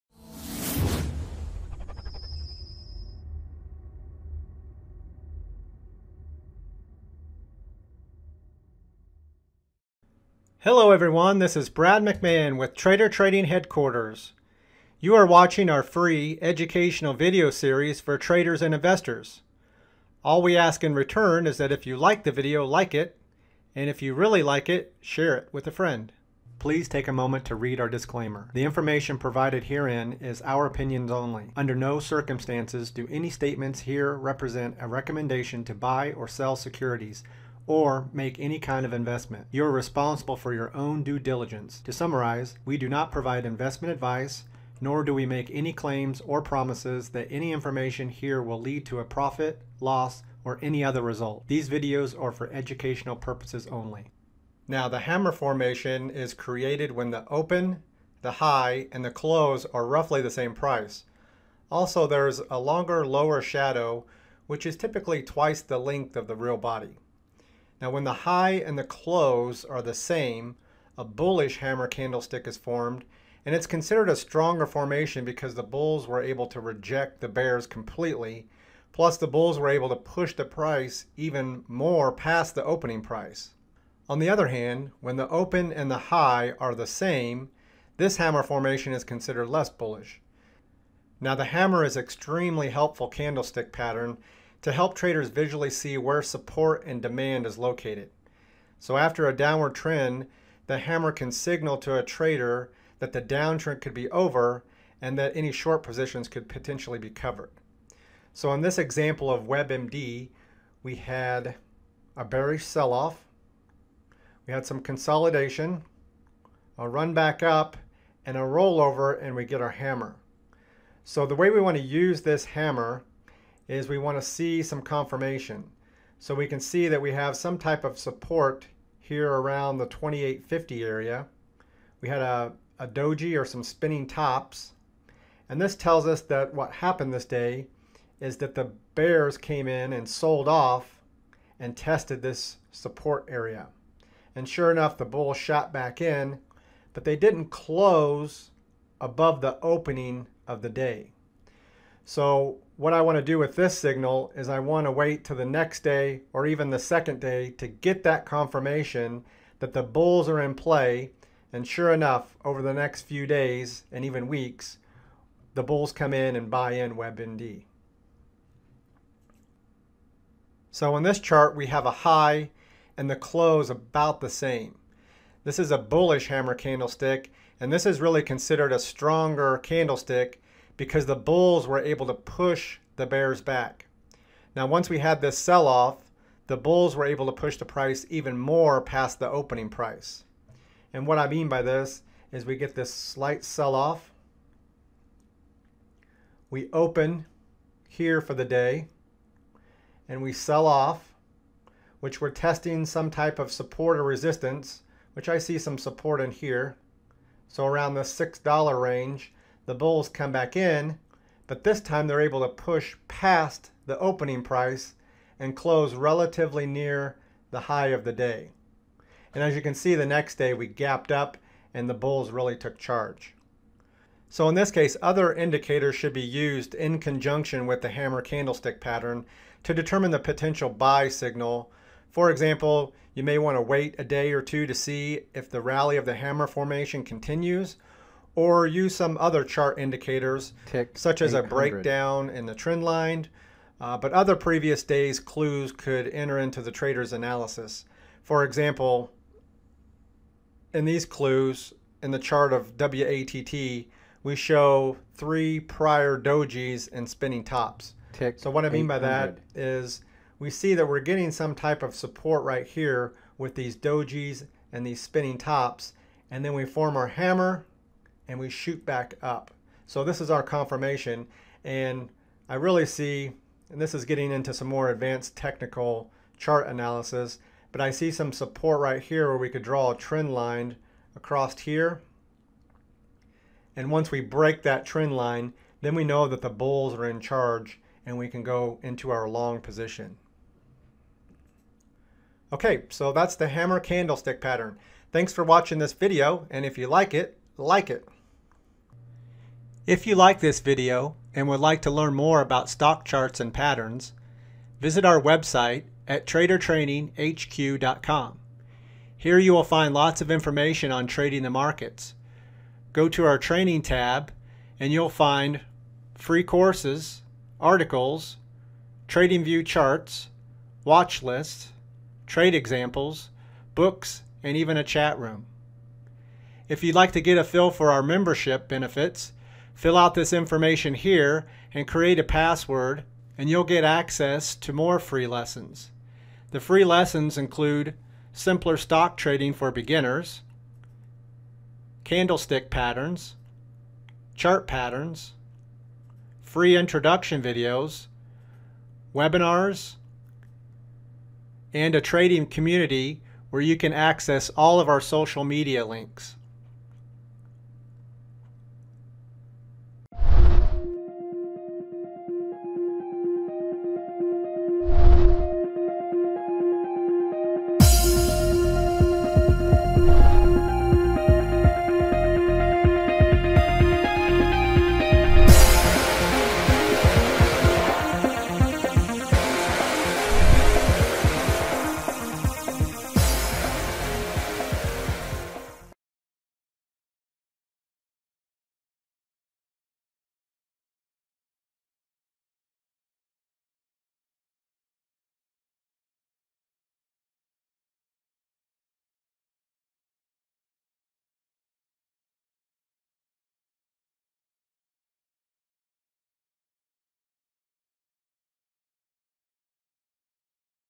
Hammer
A Hammer candlestick is formed when the price moves lower after the open, and then comes back to close above the intraday low and near the open.

If you'd like to learn more chart patterns or sign up for a free membership

If you'd like to learn more about Trader Training Headquarters, visit our